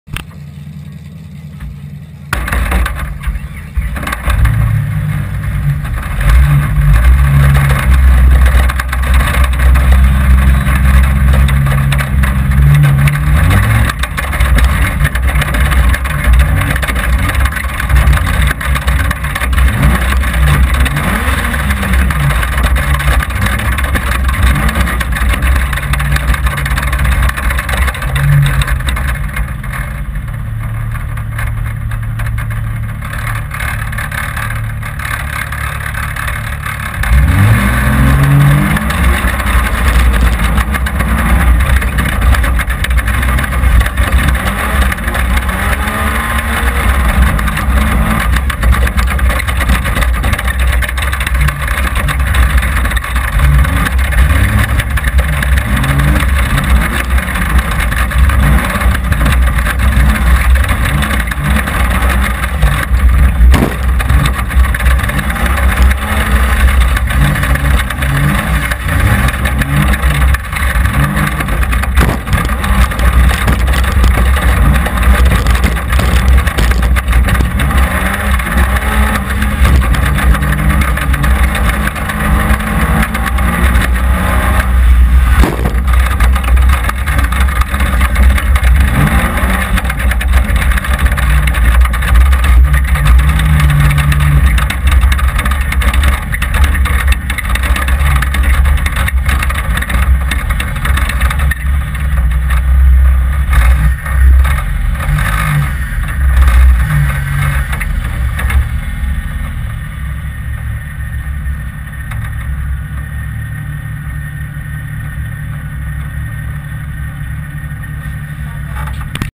knox couty tough truck on board 1
This is the first run around a very tight tough truck course at the knox county fair.